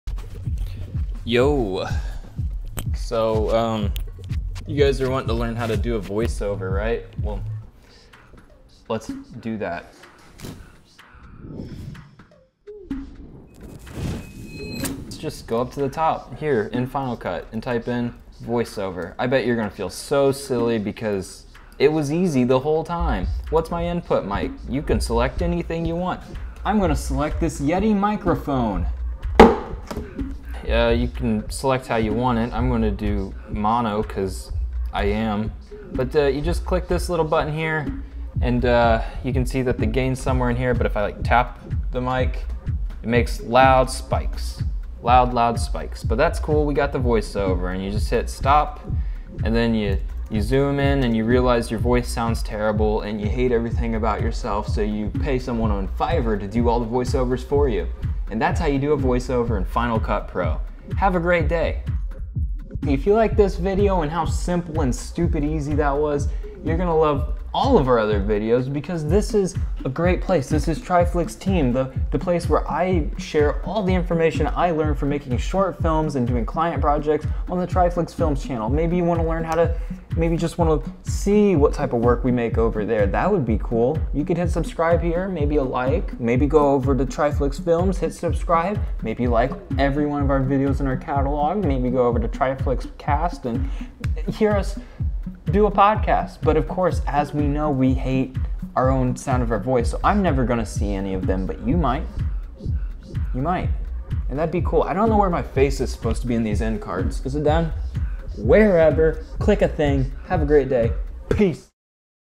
Easy Voice Over | Final Cut Pro
Voice overs can be a great way to narrate your videos. Luckily if you are using Final Cut Pro it is super easy!